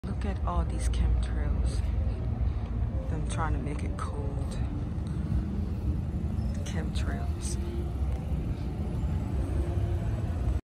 Chemical trails to make it cold
Why are they doing this? The cold is bad for everything and everyone.. it kills plants and it hurts our bodies causing arthritis.